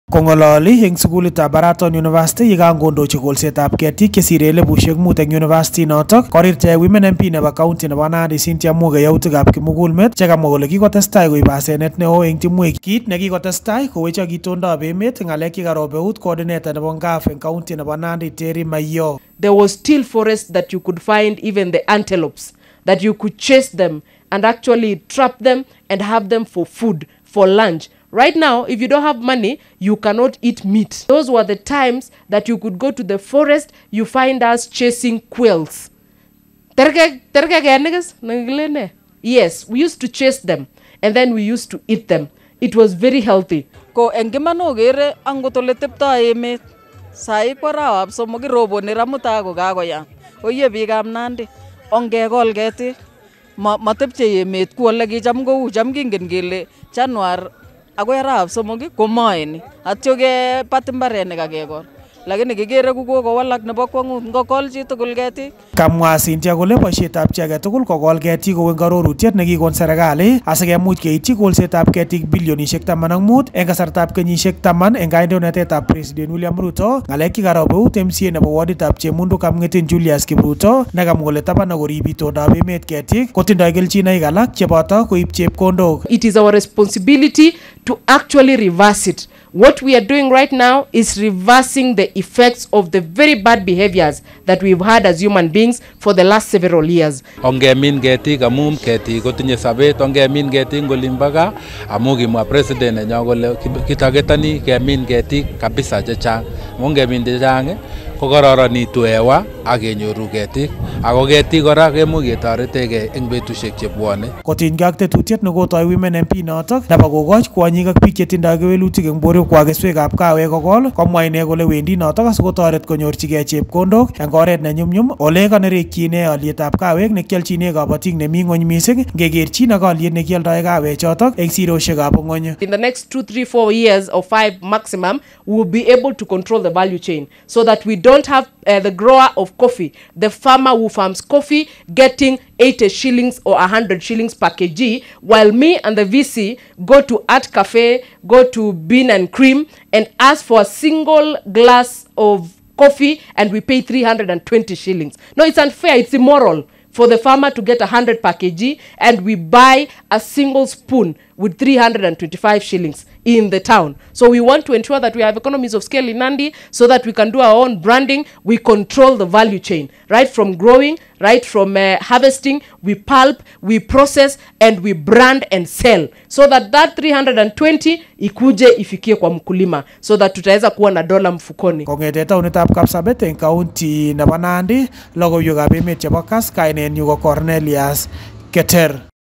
Tree planting exercise in Baraton, Nandi County
Kakool stakeholders cheterter ketiik 5000 eng sugulitab barak nebo Baraton, County nebo Nandi.